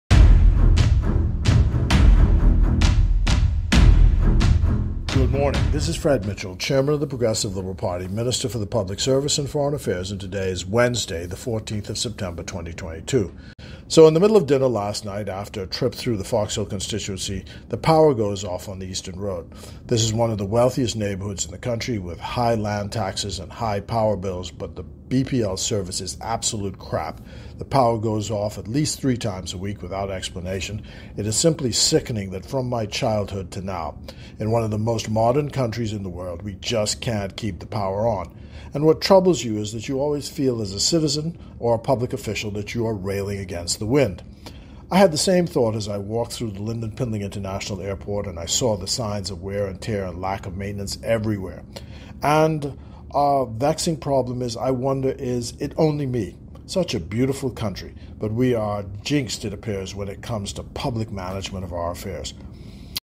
One year later, the power can’t stay on but it’s costing You!
One year later, the power can’t stay on but it’s costing you… because the PLP forgot to keep agreements the FNM put in place.